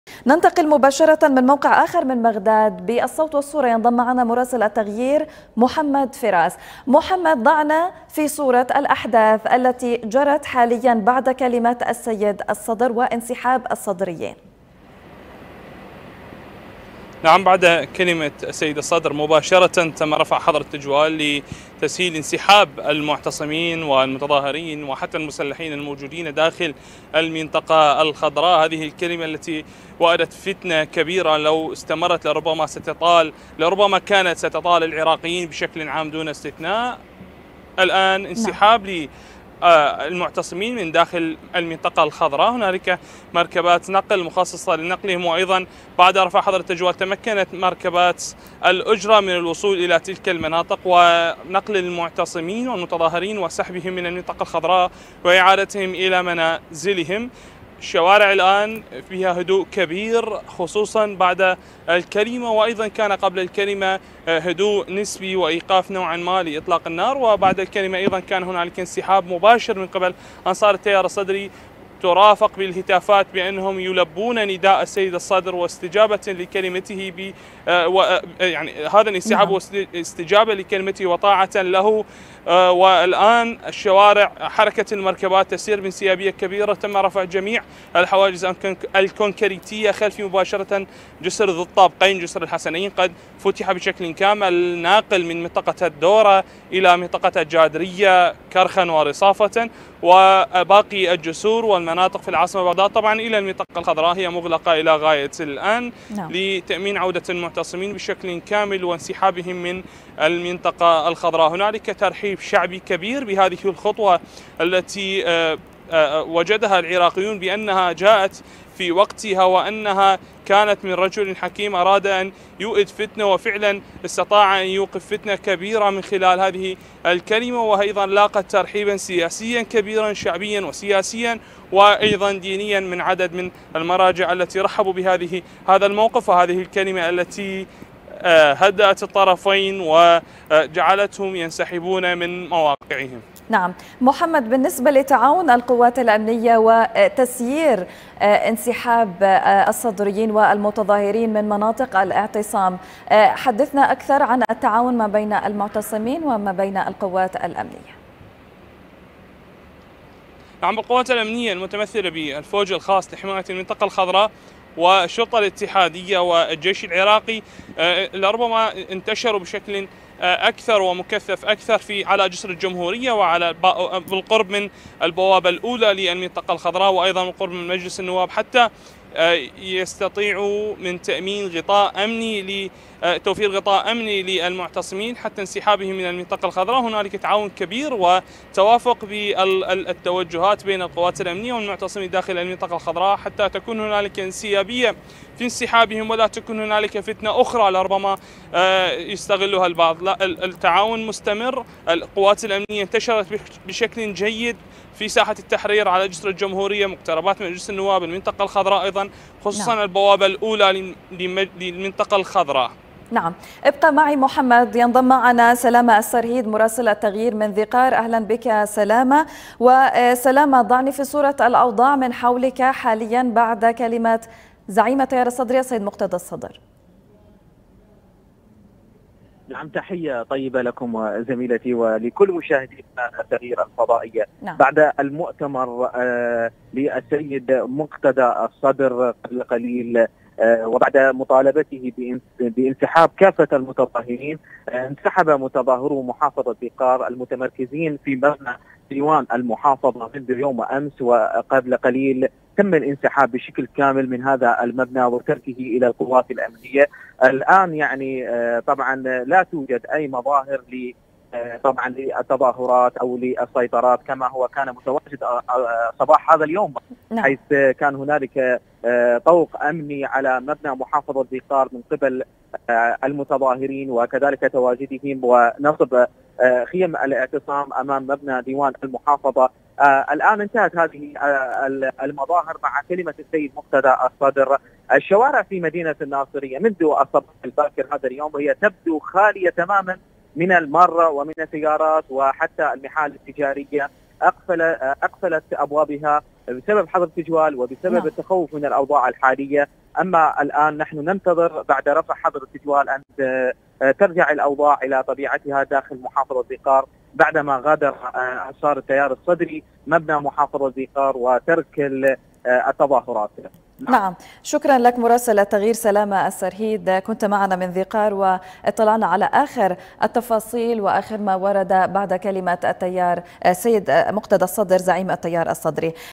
شاهد لحظات الانسحاب وآخر المستجدات في بغداد وذي قار بعد كلمة مقتدى الصدر
شاهد لحظة الانسحاب وآخر المستجدات في بغداد وذي قار  بعد كلمة مقتدى  الصدر

موقع قناة التغيير الفضائية الالكتروني